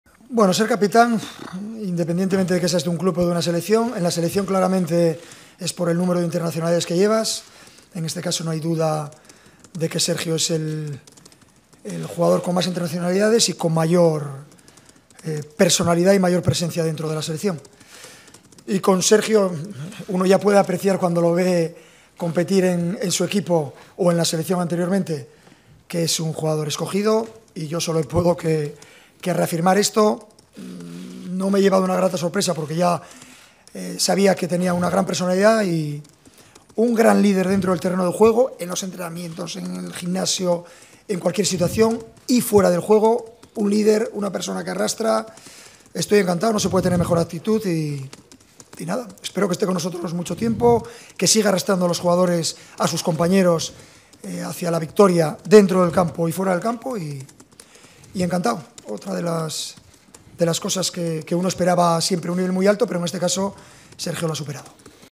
Luis Enrique, feliz con la aportación de Sergio Ramos
😍 Para los que dicen que la relación entre Luis Enrique y Sergio Ramos no va bien... 🔥🔥🔥